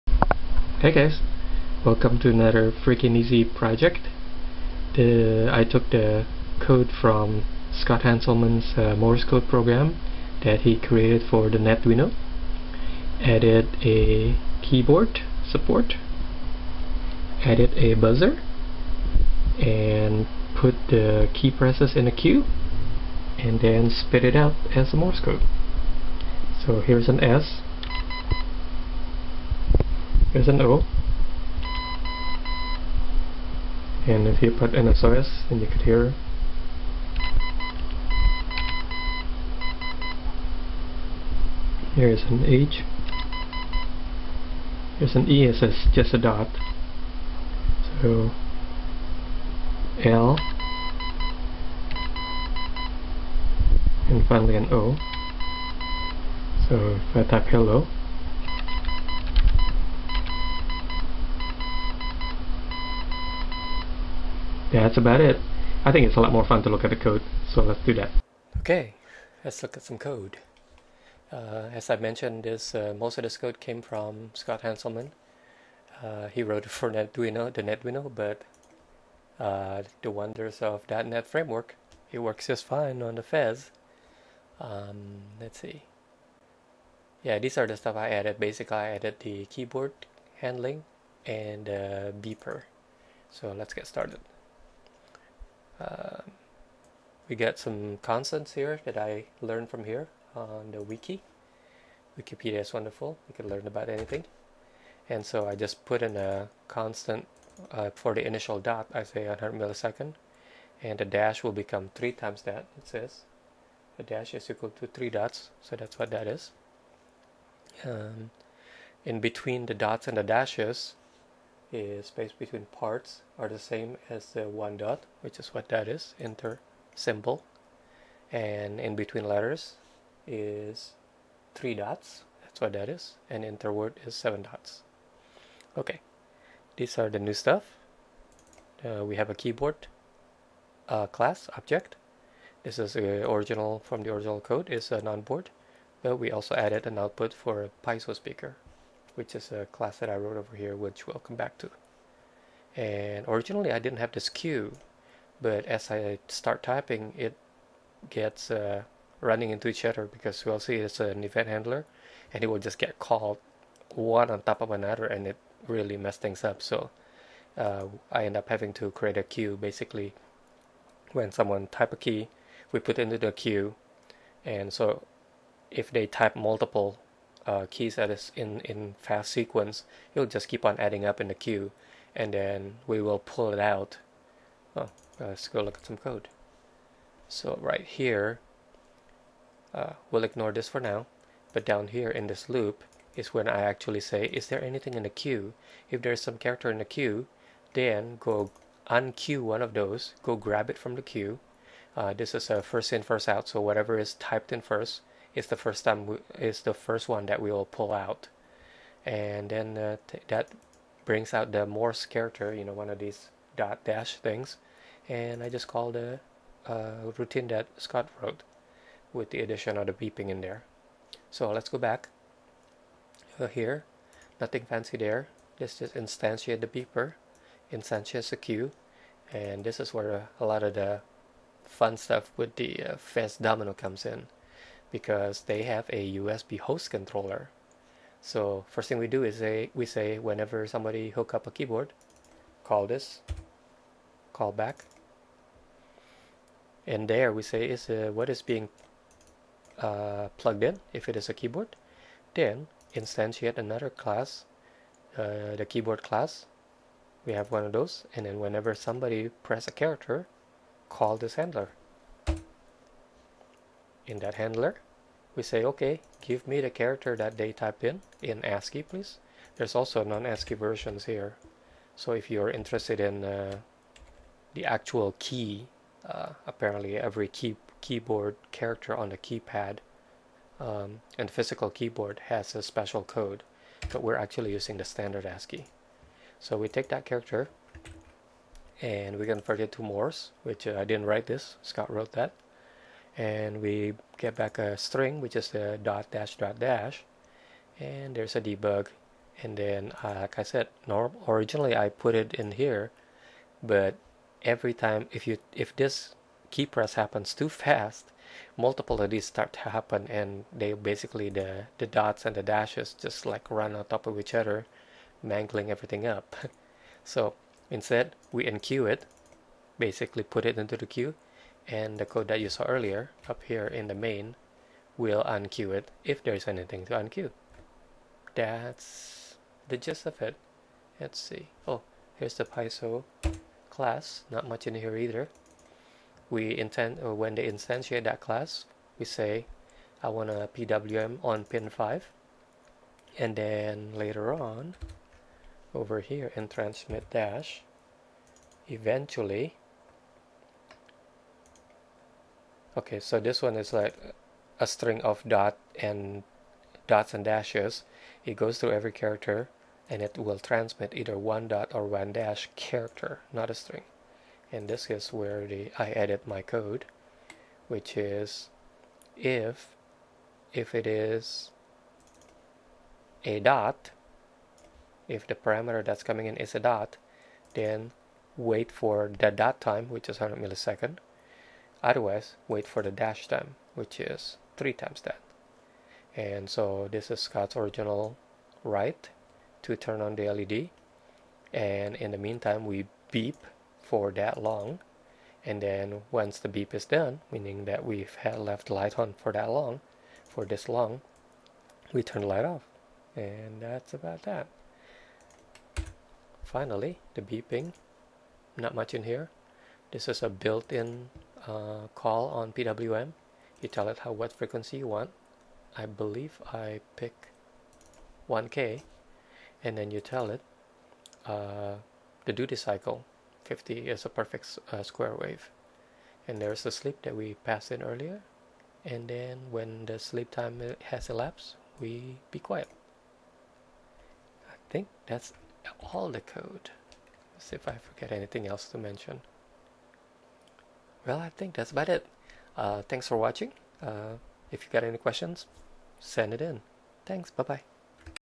FEZ Domino USB Morse Keyboard
Adaptation of Scott Hanselman's Morse Code program.  He wrote it for Netduino, but thanks to the standard .NET Microframework, it run just fine on FEZ Domino.  I added keyboard and beeper for more fun.